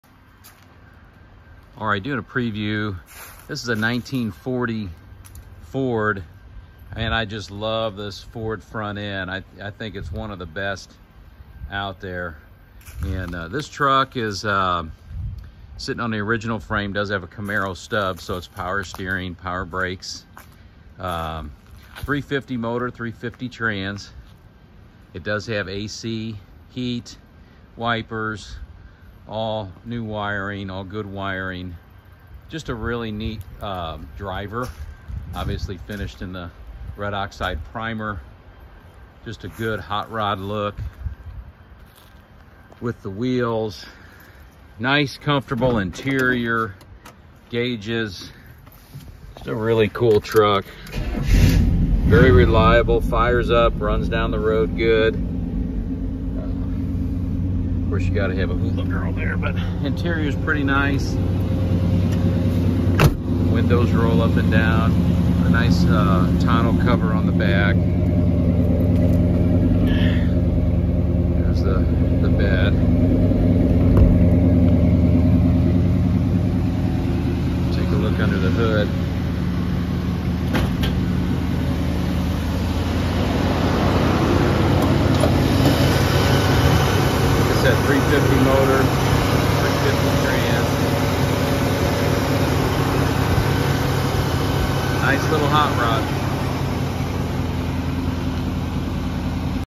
SOLD 1940 Ford Pickup Street Rod Hot Rod Shop Truck
1940 Ford Pickup Truck 350 sbc Engine 350 Automatic Transmission Camaro Subframe Power Steering Power Brakes, Disc, Drums Vintage Air, Heat, Wipers 314/498/0950